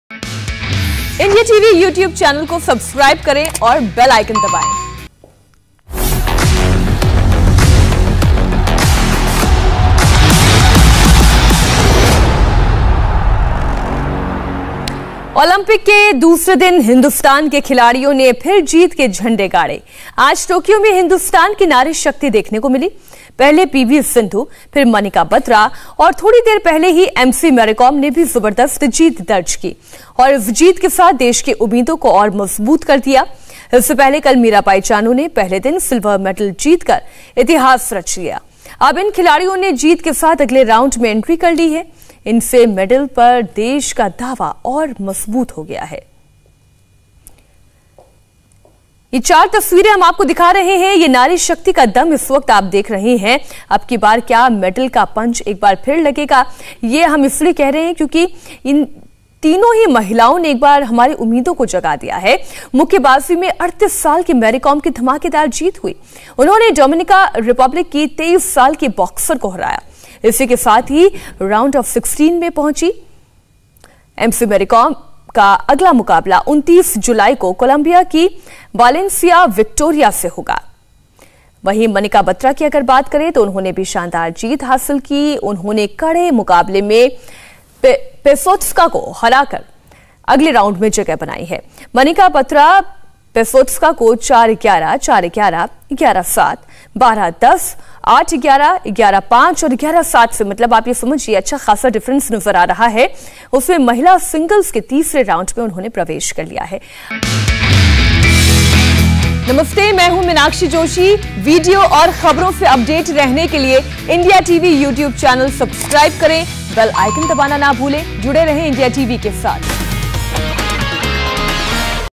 Indian Women Athletes Backbreaking At Tokyo Olympics, India Cheers For Sindhu, Mary Kom And Manika
Boxer Mary Kom and Shuttler PV Sindhu have cleared for the next rounds at the Tokyo Olympics, on Sunday. Meanwhile, Manika Batra has also cleared for the next round at the Table Tennis event.

On Saturday, Mirabai Chanu had won a silver medal at the weightlifting event. Thus making a historic beginning for India at Tokyo Olympics.